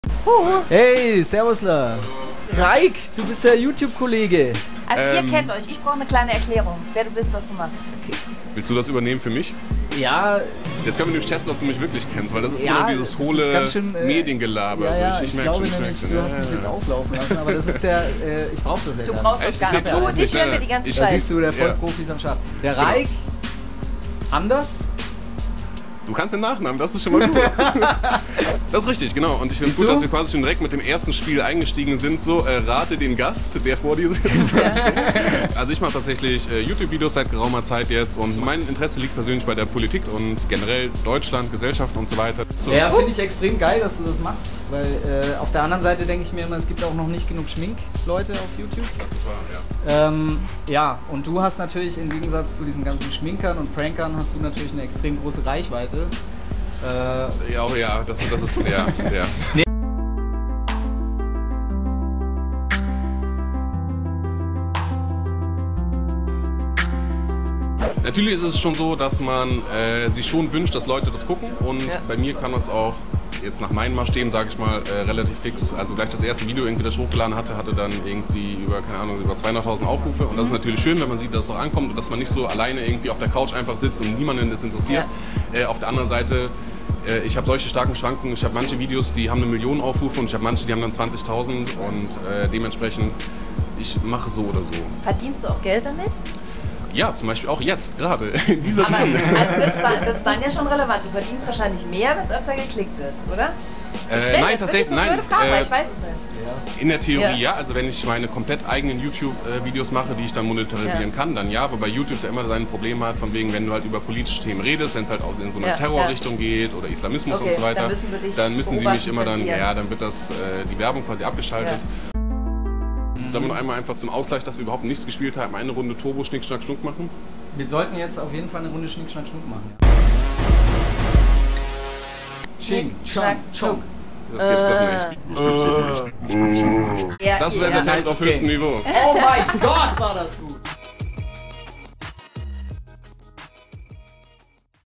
Rayk Anders vs. Phil Laude (Y-TITTY): Interview | SWR
Das Interview wurde gedreht im Rahmen des "SWR Fernsehen Familienfests"